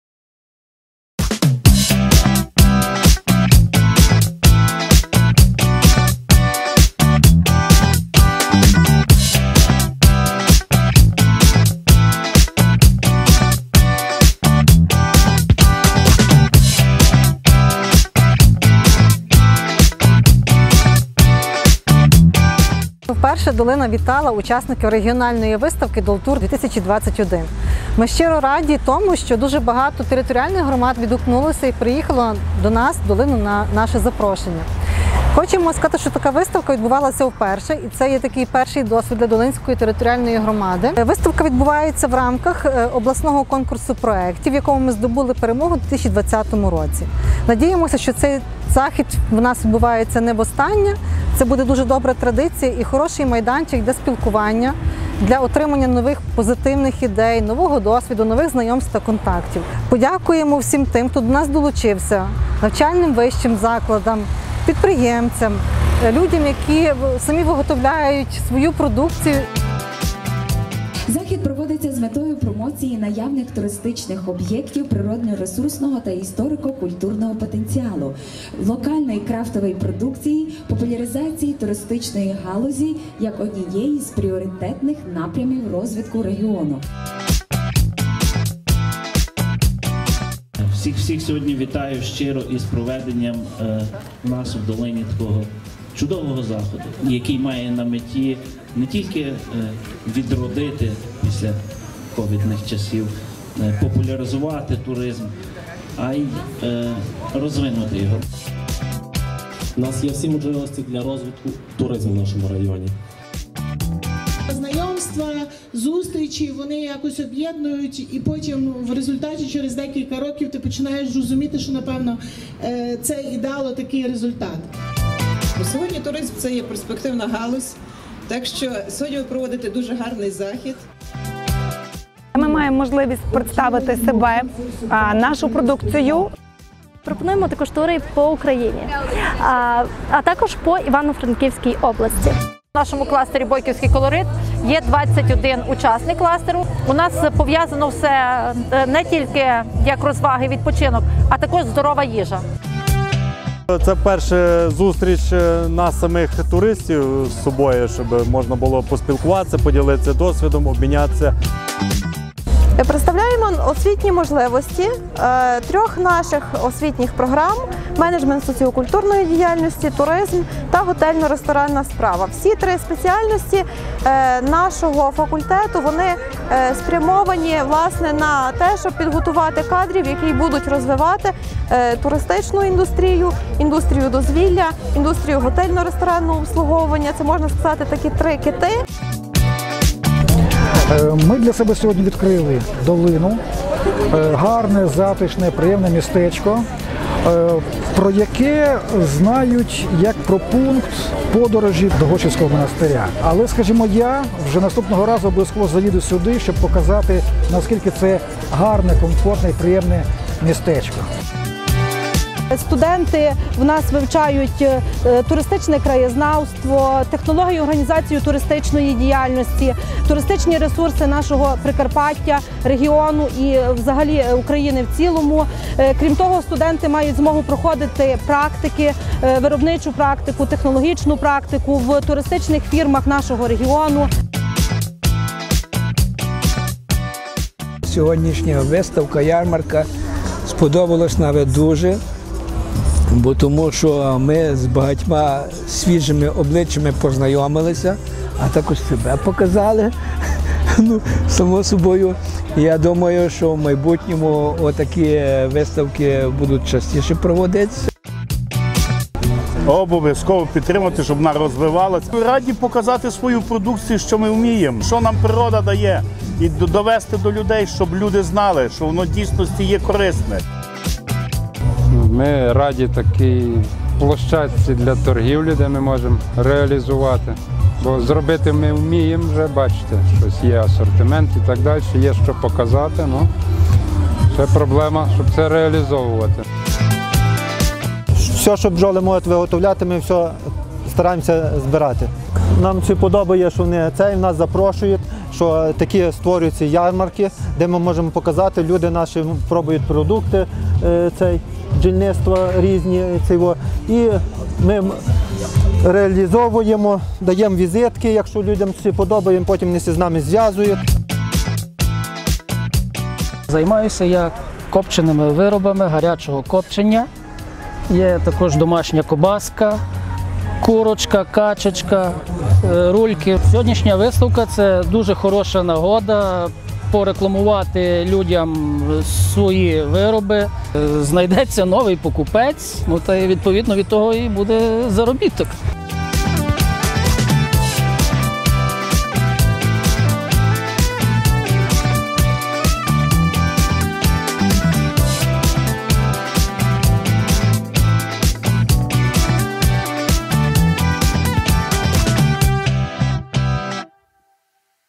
Долина вперше вітала учасників регіональної туристичної виставки-ярмарку
30 вересня в Долині відбулось відкриття Першої регіональної туристичної виставки-ярмарку “DOLTOUR-2021”. На запрошення відгукнулись учасники з сусідніх територіальних громад, які представляли туристичні можливості, послуги та крафтову продукцію.